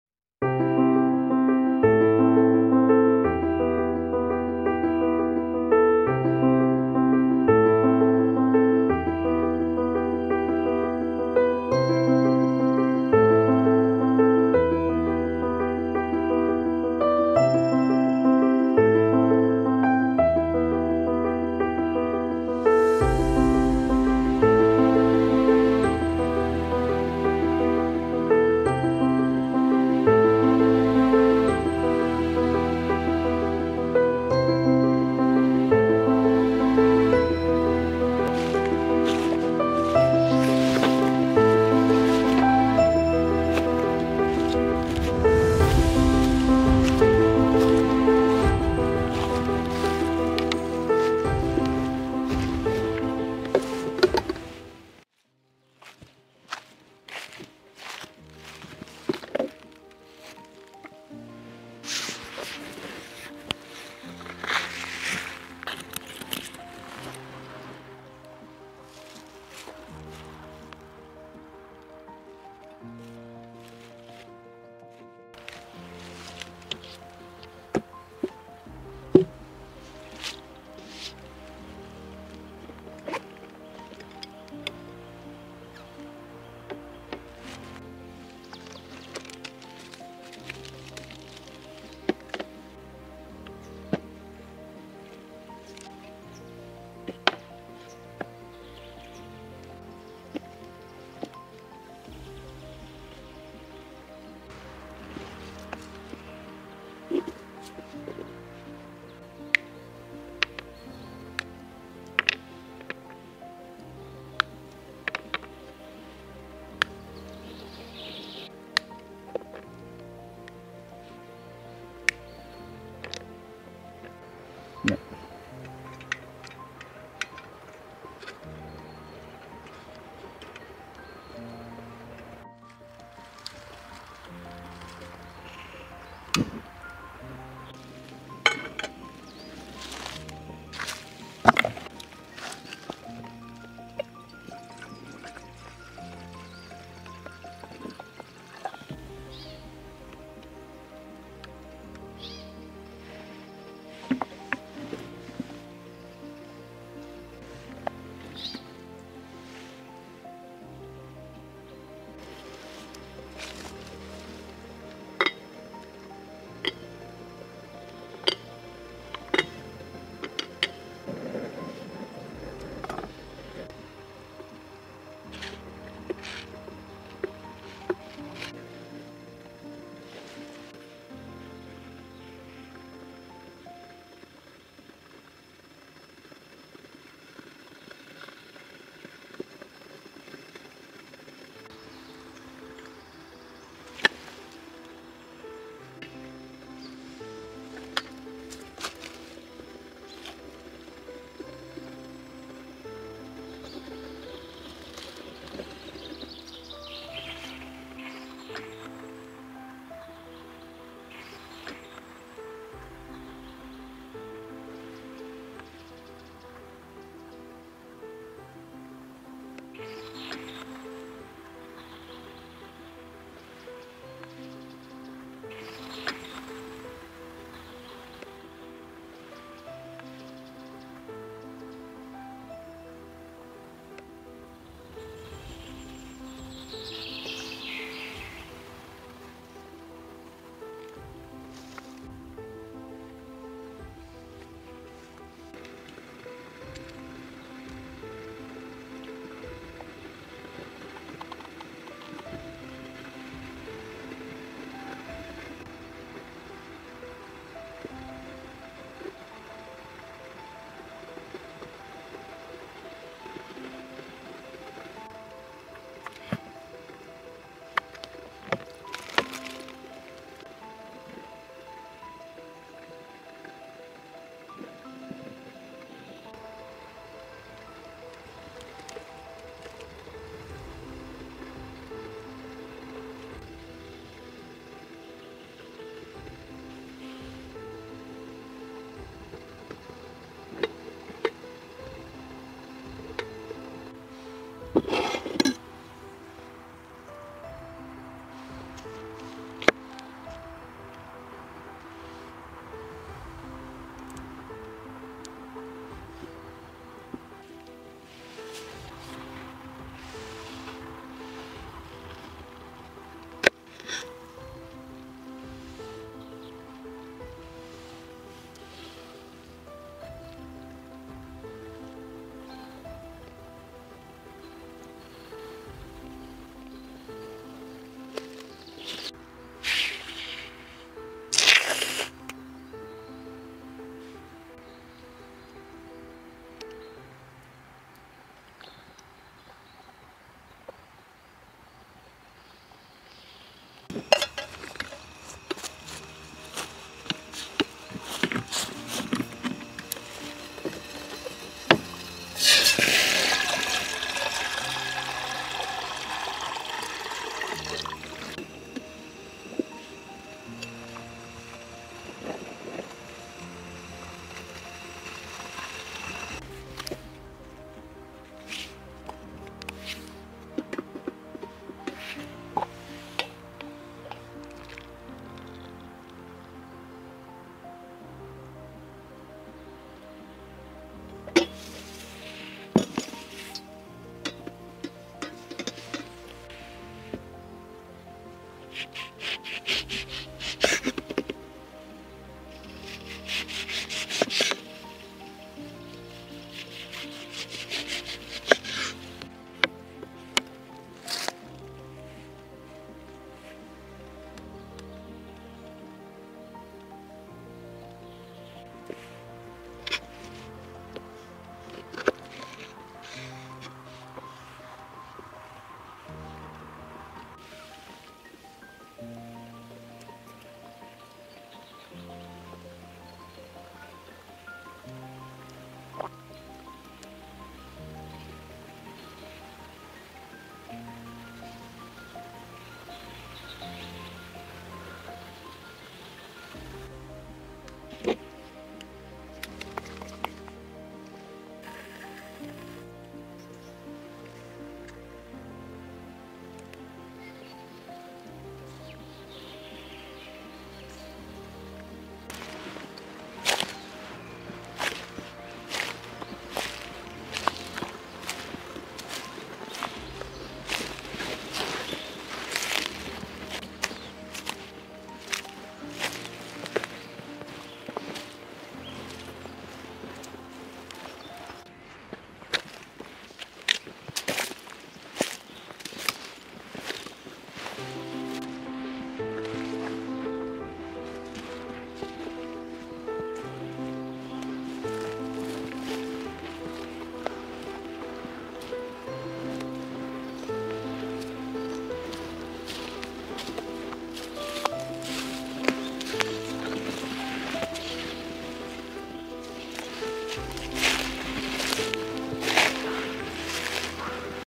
Solo camping. Spring mood. Breakfast in the forest with strawberries 🍓🍓🍓
Today I went for a walk in the forest in search of spring mood. I brought strawberries, cream and tea to enjoy.
I quickly melted the chocolate and put the strawberries in it.
No Copyright Vlog Music for Video
Also another solo camping video 🥰